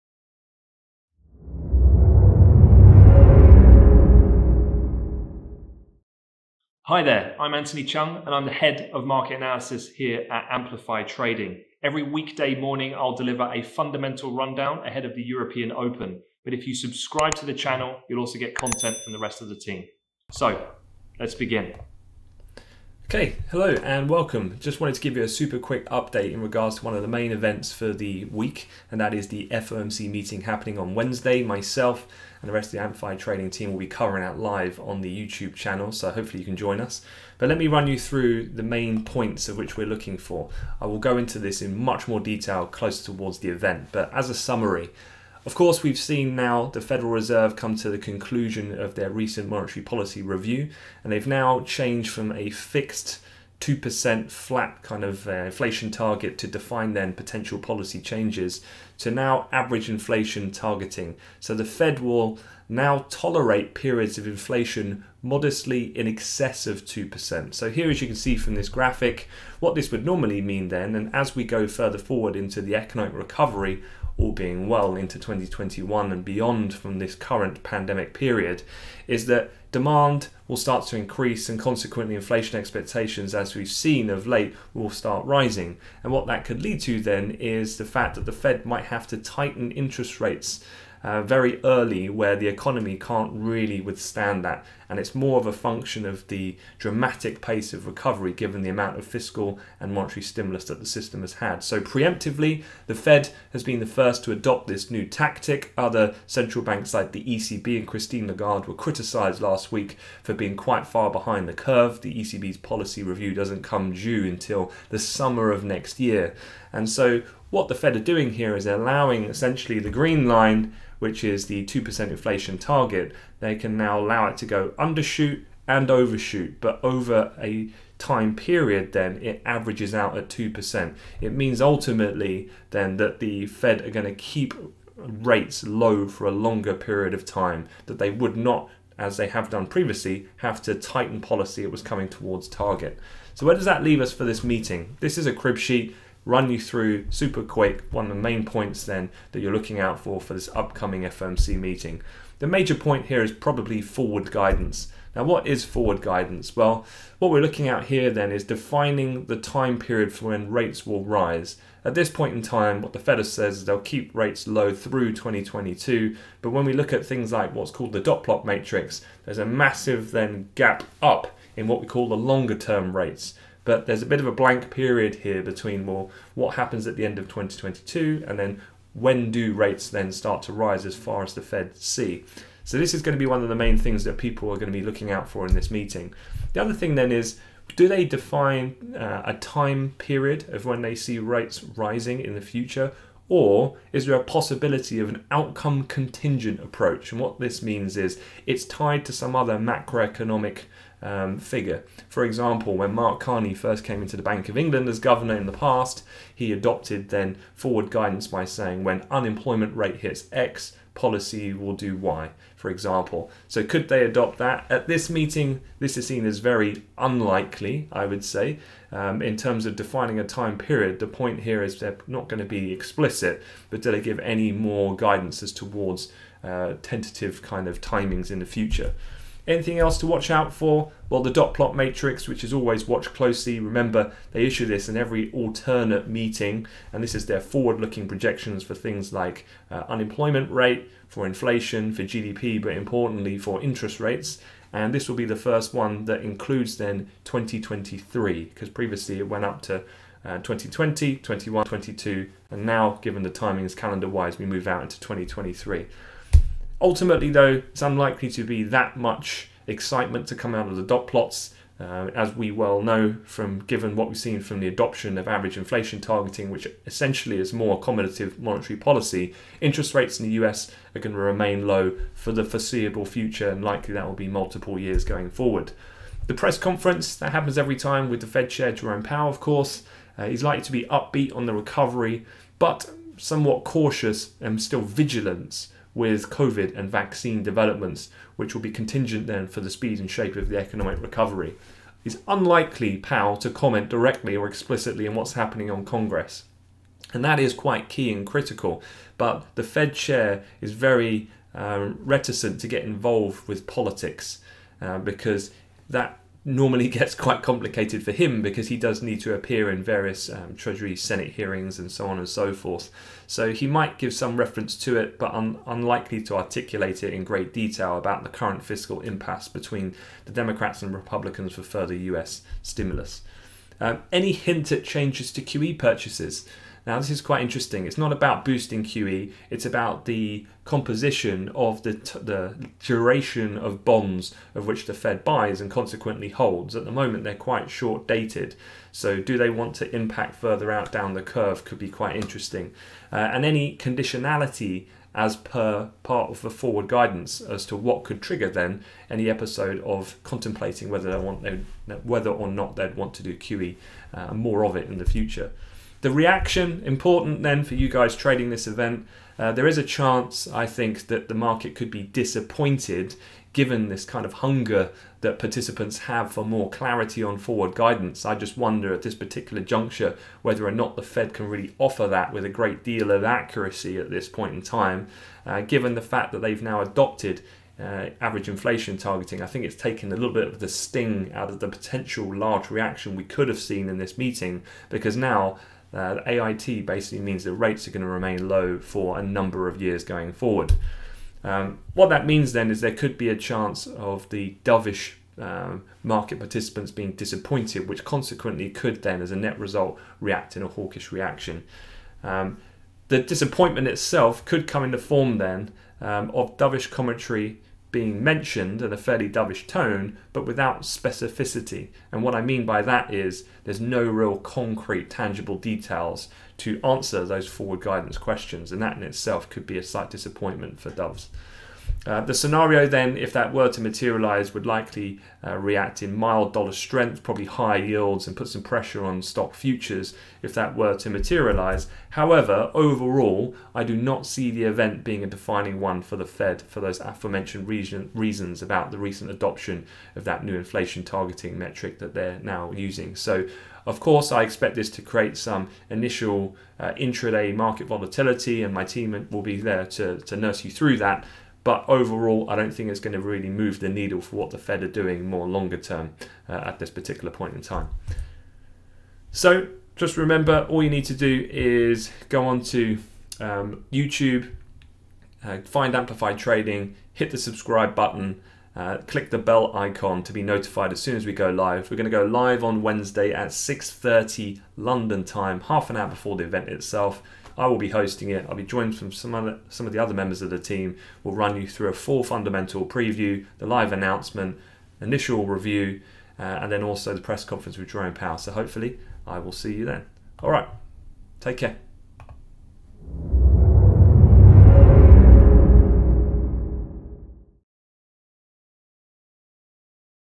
FOMC Preview: What You Need To Know
Head of Market Analysis Anthony Cheung gives a short concise preview on what to expect from the FOMC meeting on the 16th Septemebr 2020.